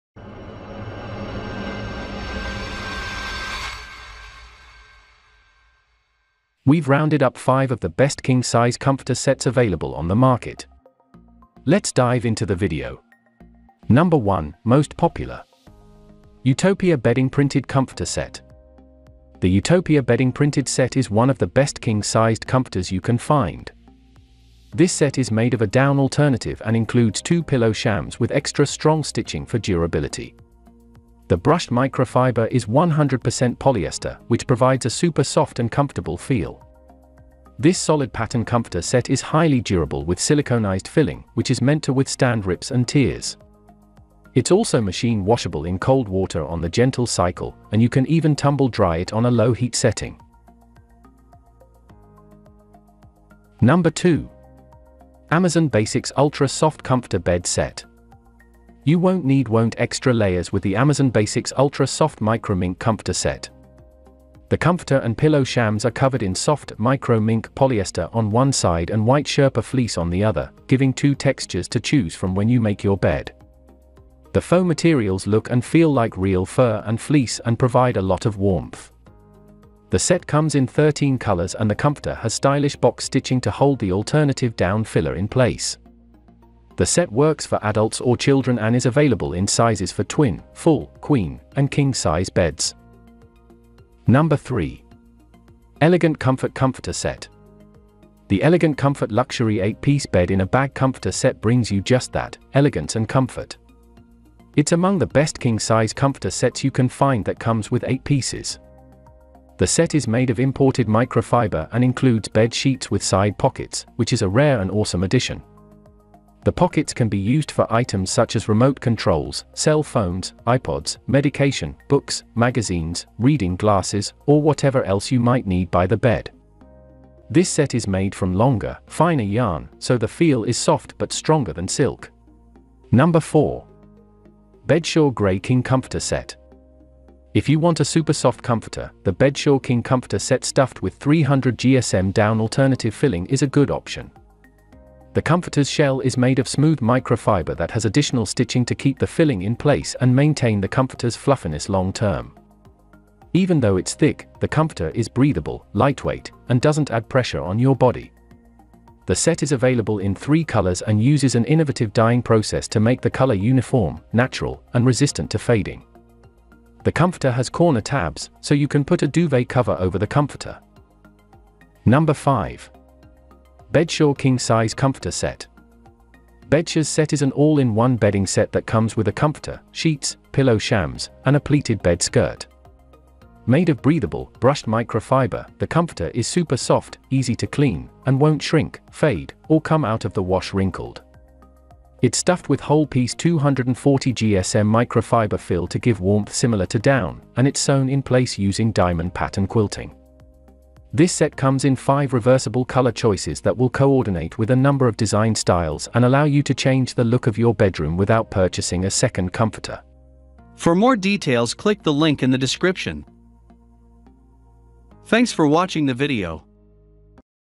Best King Comforter Set In 2023 - Top 5 Review | Soft and Comfortable Machine Washable Comforter
Links to the best king comforter set listed in this review video:
► 1. Utopia Bedding Printed Comforter Set
► 2. Amazon Basics Ultra-Soft Comforter Bed Set
► 3. Elegant Comfort Comforter Set
► 4. Bedsure Grey King Comforter Set
► 5. Bedsure King Size Comforter Set

🕝Timestamps🕝
0:06- Introduction

0:15 - Utopia Bedding Printed Comforter Set
0:54 - Amazon Basics Ultra-Soft Comforter Bed Set
1:33 - Elegant Comfort Comforter Set

2:12 - Bedsure Grey King Comforter Set
2:51 - Bedsure King Size Comforter Set

★What is a king comforter set?
Comforter sets differ from bed-in-a-bag sets in that they don't contain sheets and pillowcases. A basic comforter set is sold with a comforter and two decorative pillow shams. A deluxe comforter set may also include a bed skirt, additional decorative shams and several decorative throw pillows.